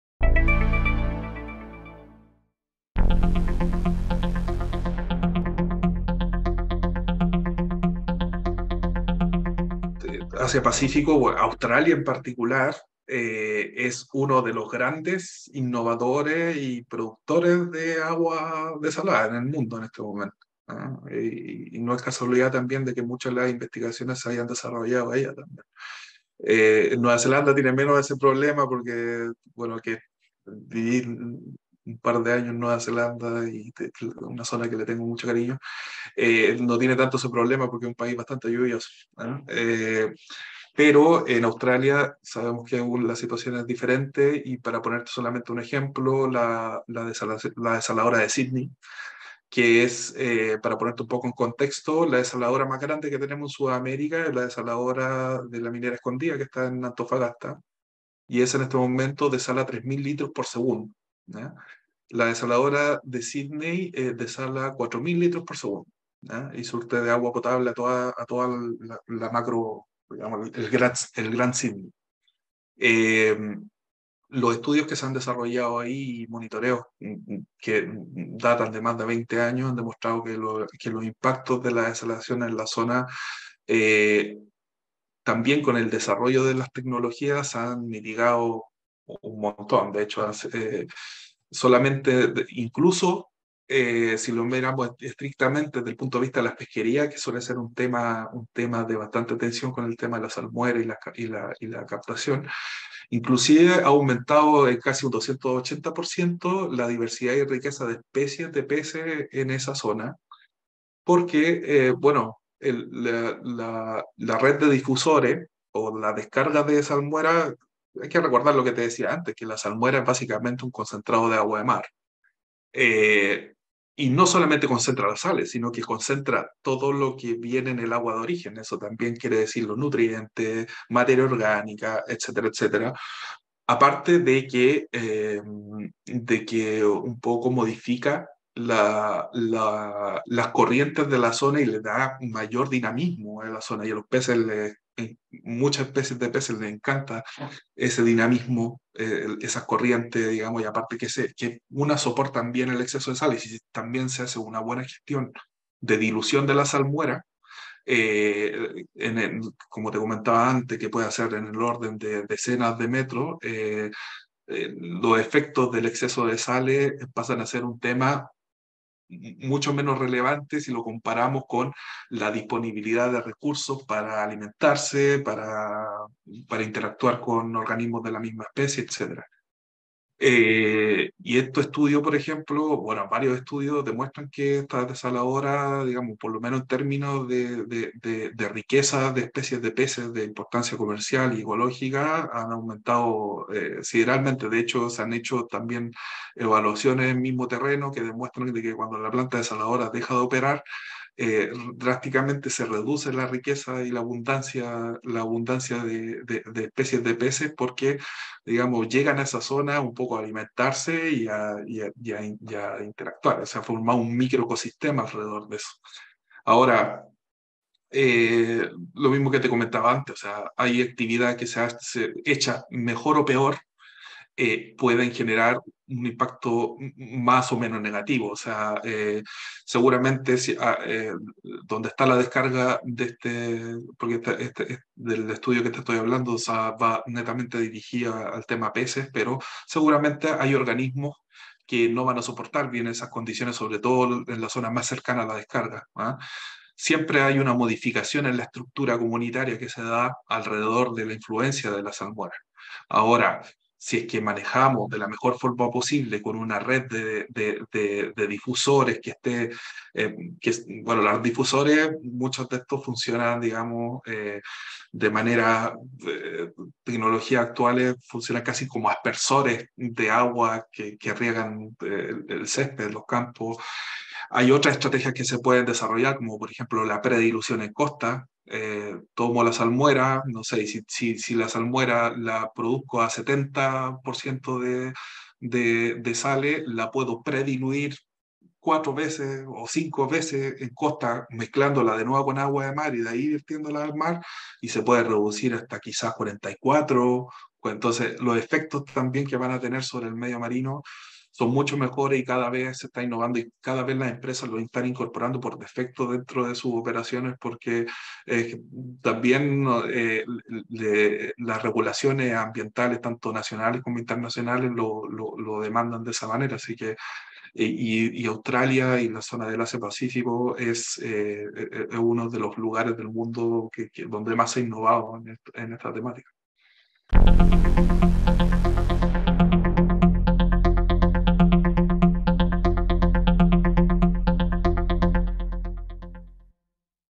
Claudio Sáez: experiencia en Australia y sus aportes a la discusión ambiental
Director del Centro Hub Ambiental de la Universidad de Playa Ancha comentó la experiencia de Australia en desalación, puntualmente la Planta de Sydney.

Más detalles de esta entrevista, en la nota publicada por el Programa Asia Pacífico de la BCN